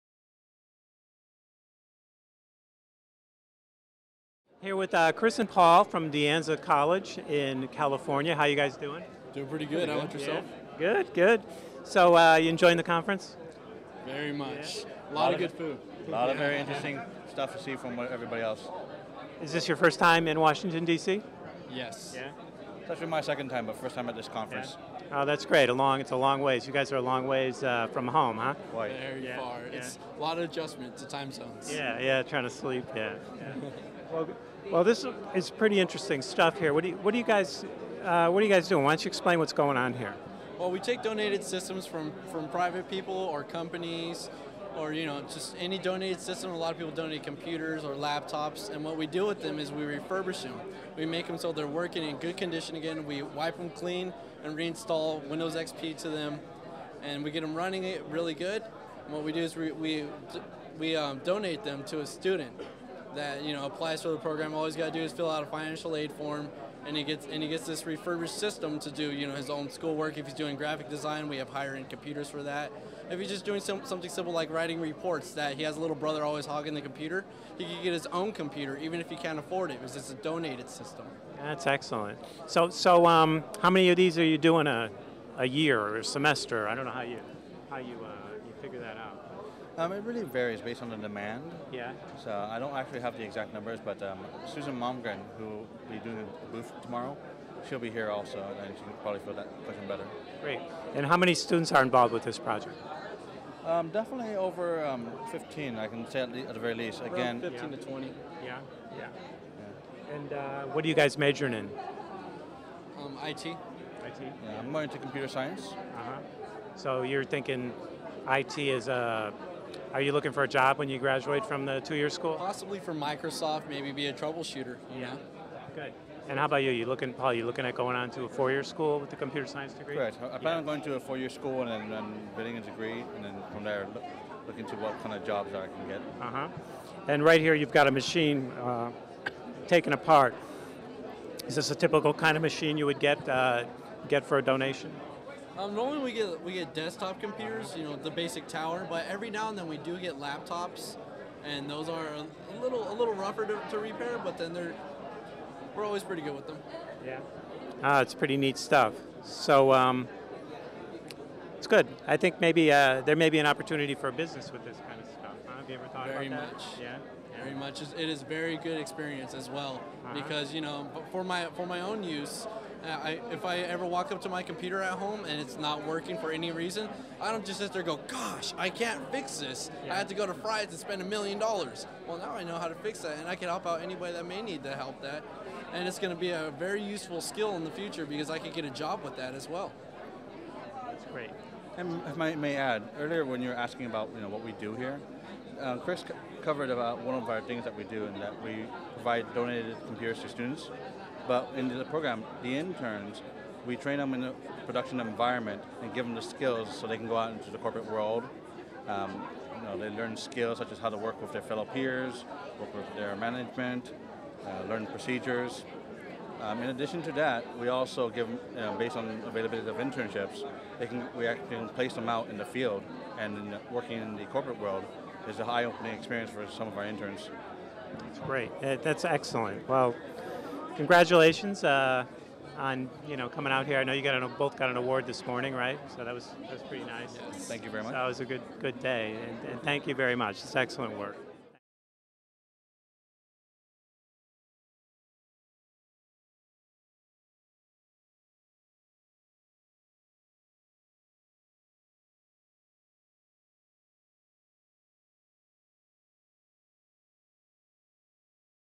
ATE 2009 Student Interview DeAnza College
Gordon has a conversation with DeAnza College students Chris Kalaveras and Paul Nguyen in October 2009 at The National Science Foundation ATE 2009 Principal Investigators Conference in Washington, DC.